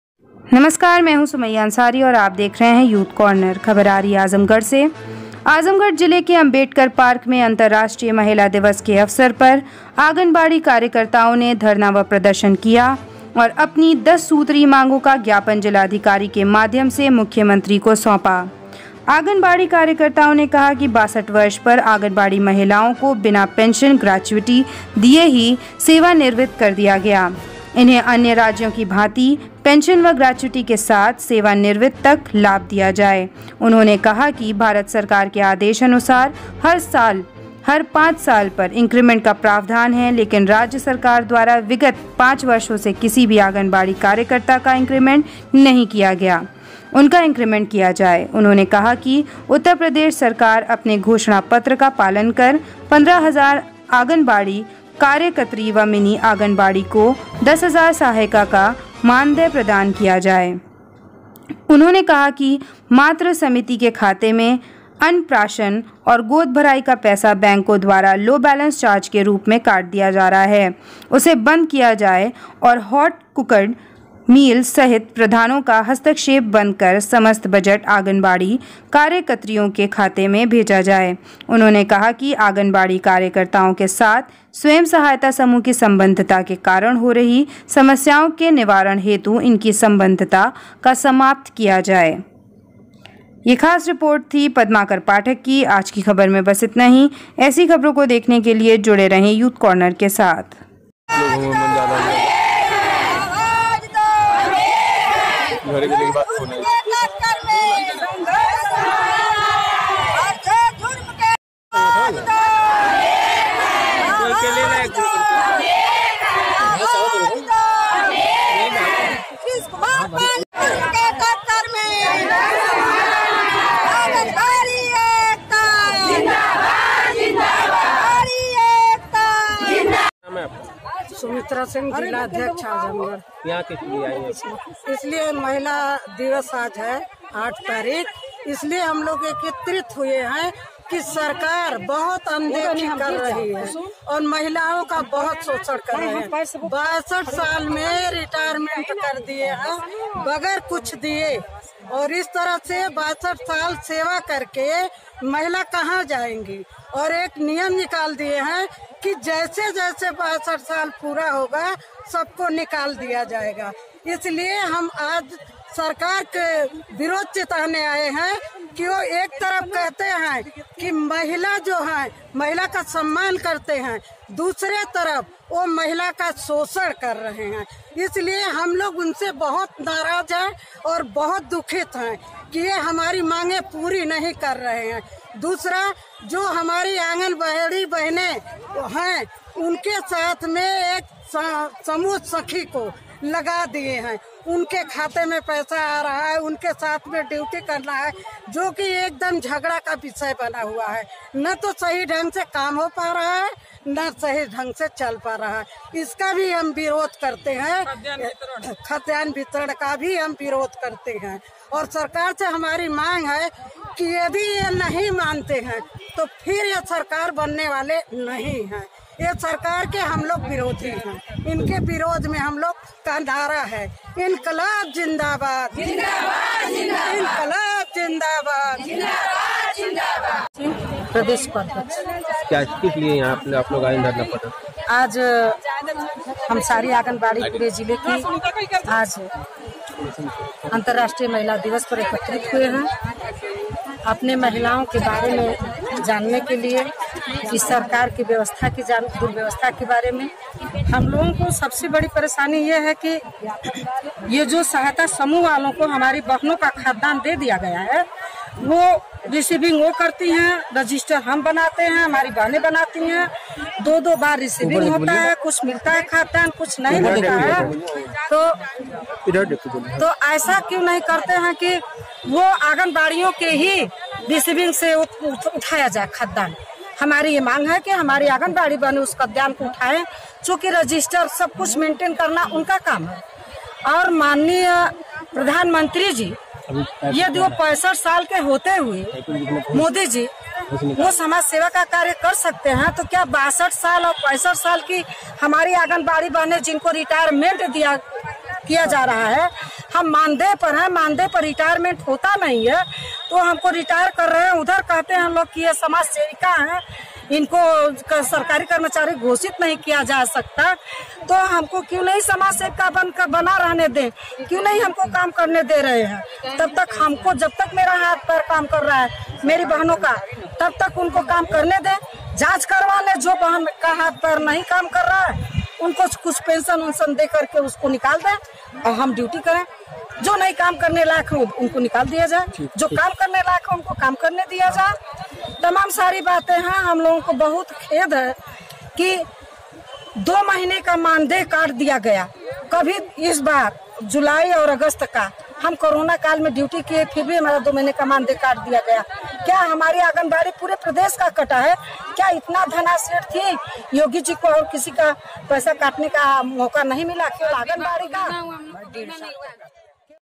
आज़मगढ़/महिला दिवस के अवसर पर 10 सूत्री मांगों को लेकर आंगनबाड़ी महिलाओ ने ज्ञापन जिलाधिकारी को सौंपा
Video from Brijesh Prajapati